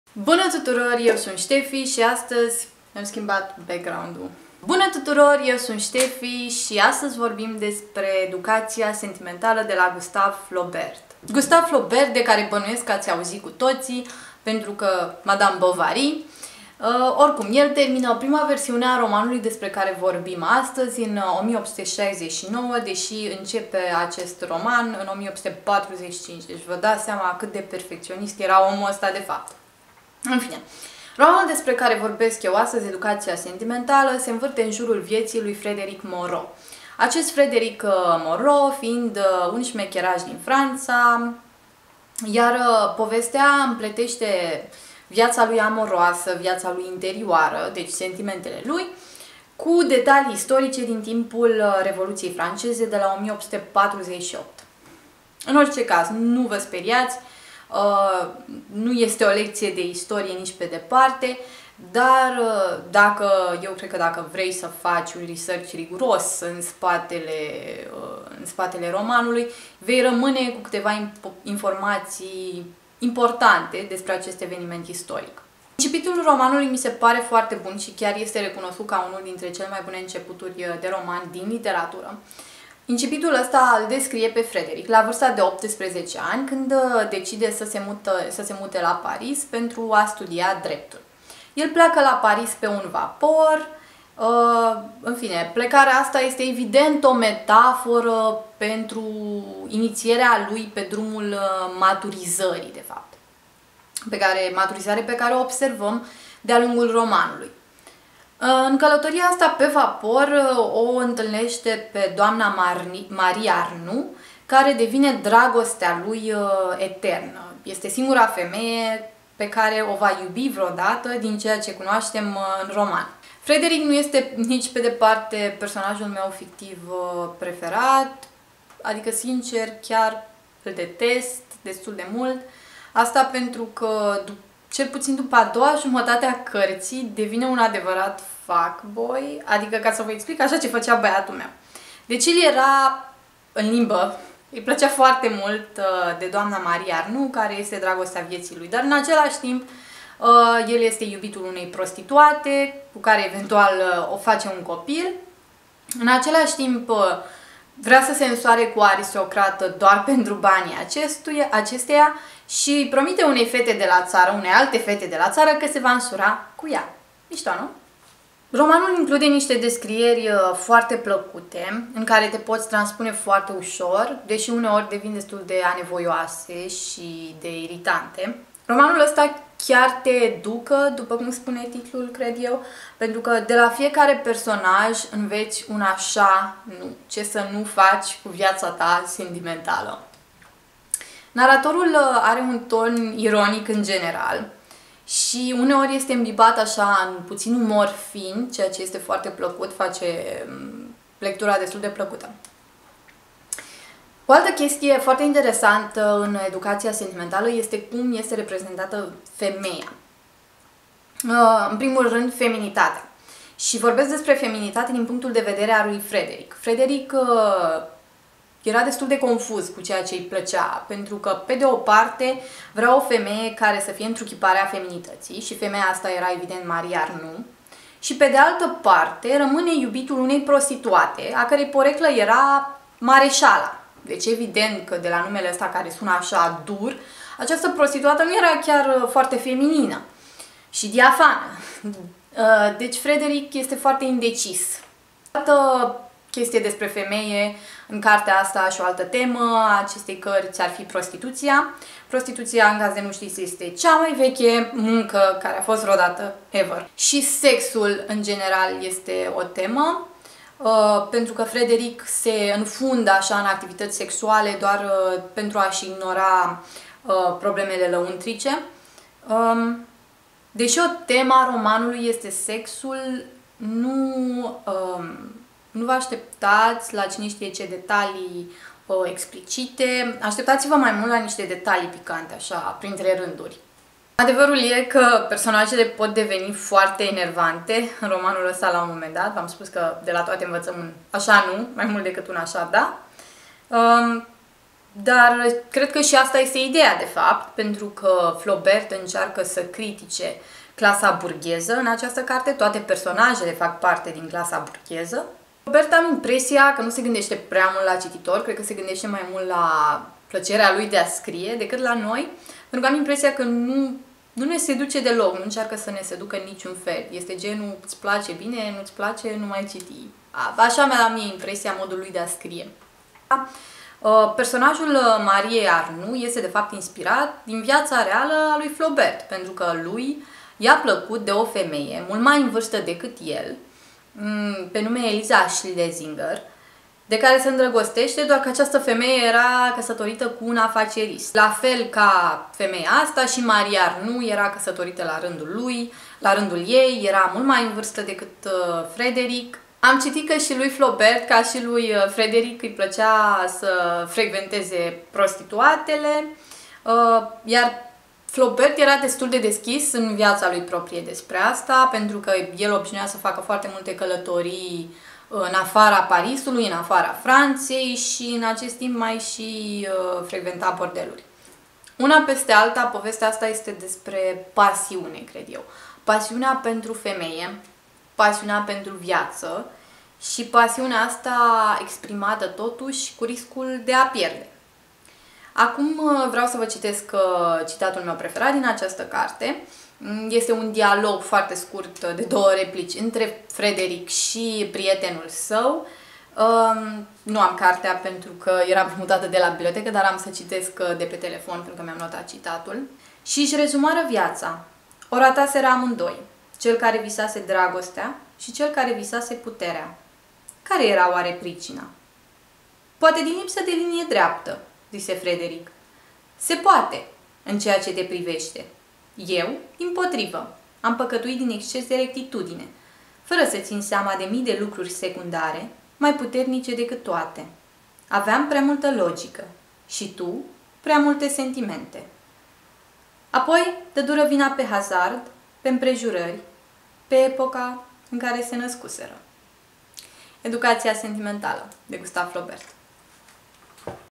Educația sentimentală - Gustave Flaubert (RECENZIE)
O poveste despre iubire și promiscuitate care urmează pașii unui tânăr indecis îndrăgostit de o femeie măritată. Tu ce crezi? Îi dai o șansă lui Flaubert?